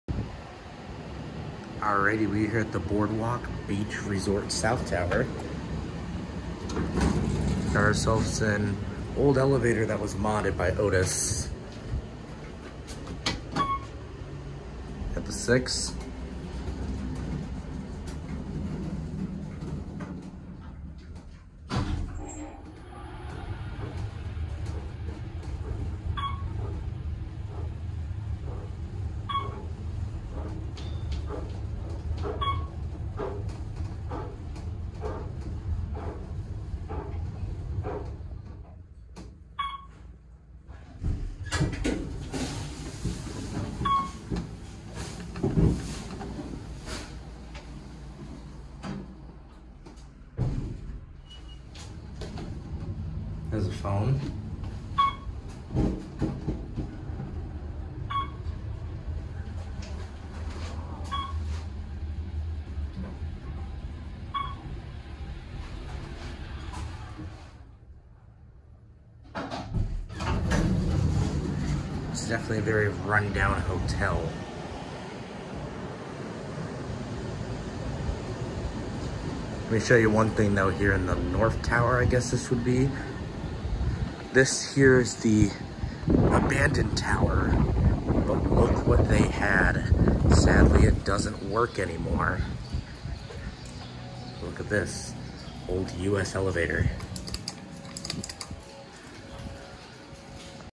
Otis Bottom- Drive Traction Elevator - Boardwalk Beach Resort South - Myrtle Beach, SC
This was something pretty old, probably an old otis but has been modernized by otis. This property is very sketchy and very creepy at night. They have a second tower that is completely abandoned and sadly has a US elevator that is now off. According to the records the US elevator in the other tower has only been shut off for about a year so I just missed it.